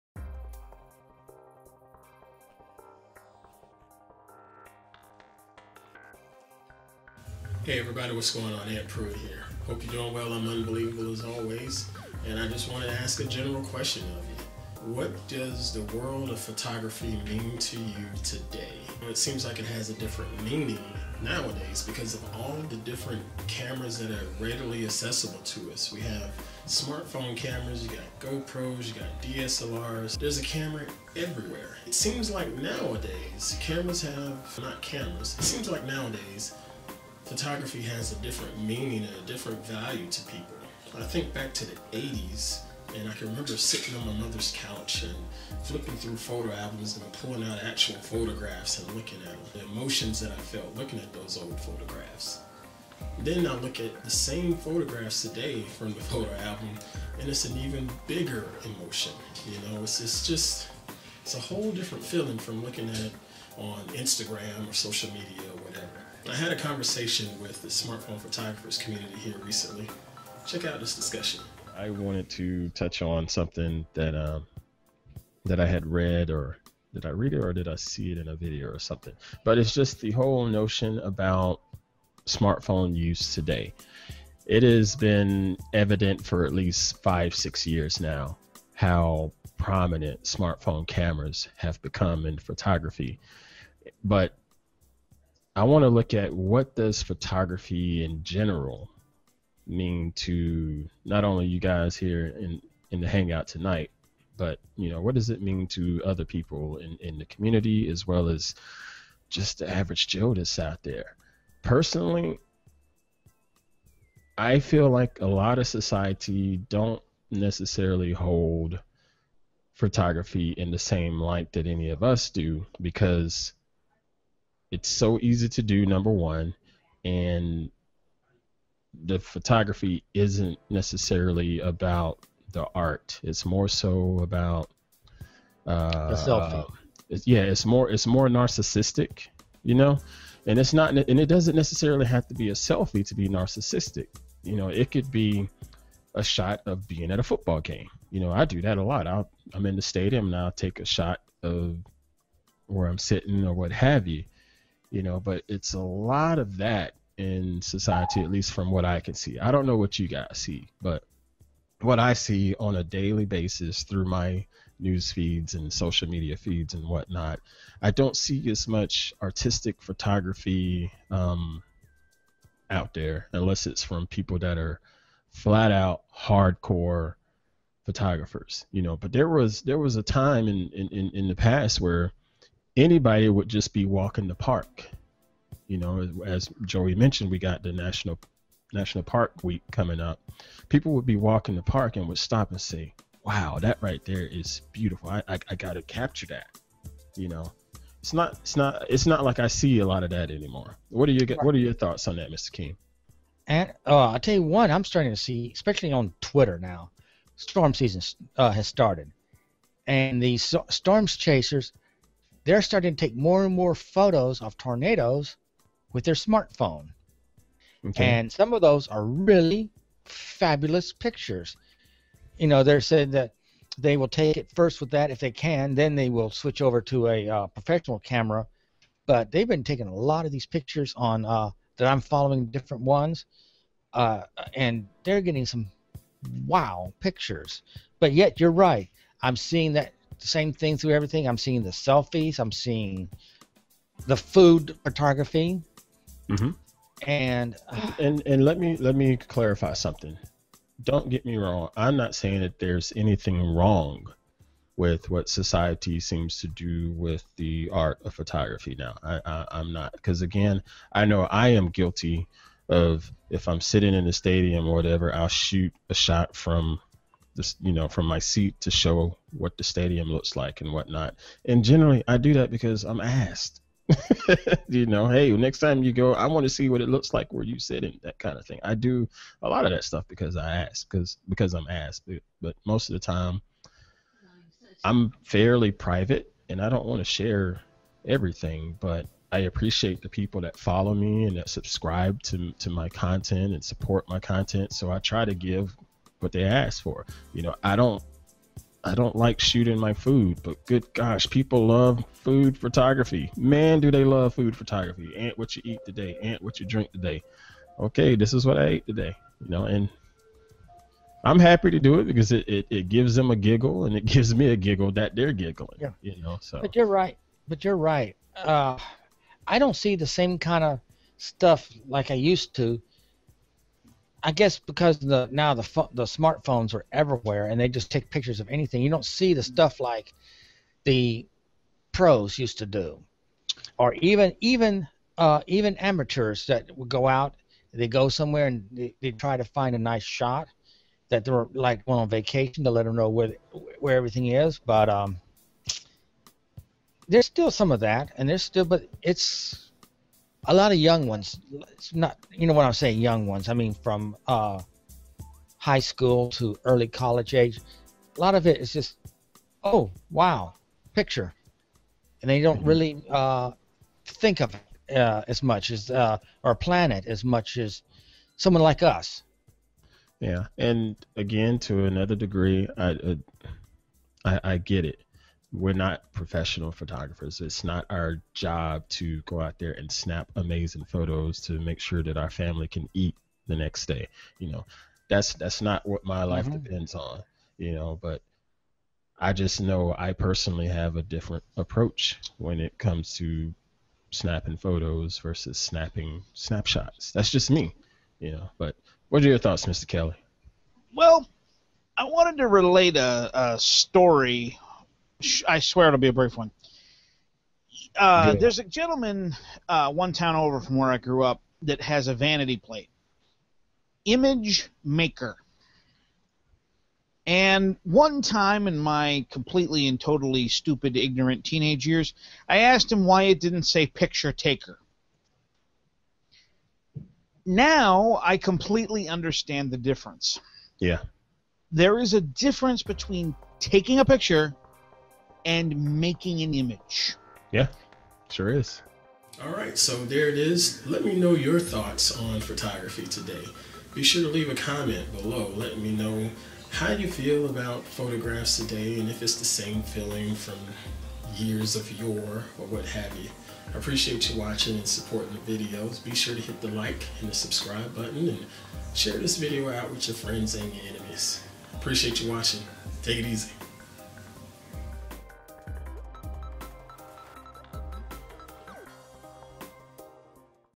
What's Your Value of Photography Today?
I ask this question to you and the panel of the Smartphone Photographers Community hangout on air.

Ant Pruitt Media, LLC